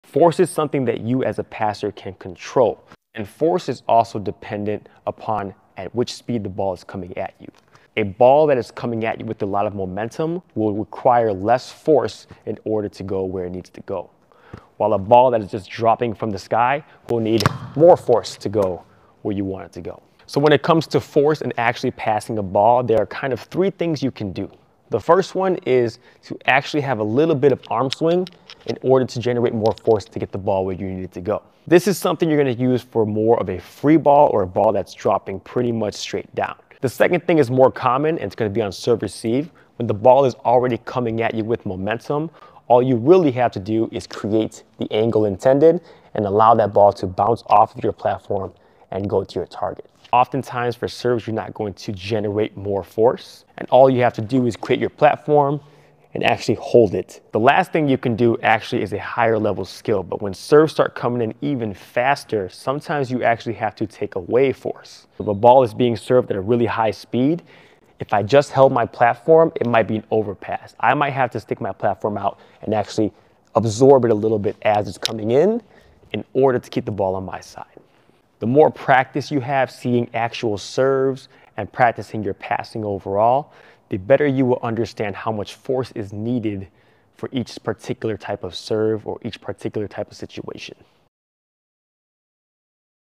FORCE and Passing in Volleyball
FORCE plays a big role in passing. Sometimes you need to add force while other times you need to absorb force. But how do you know how much force is needed?

The Answer: it depends on the speed/momentum of the ball you are passing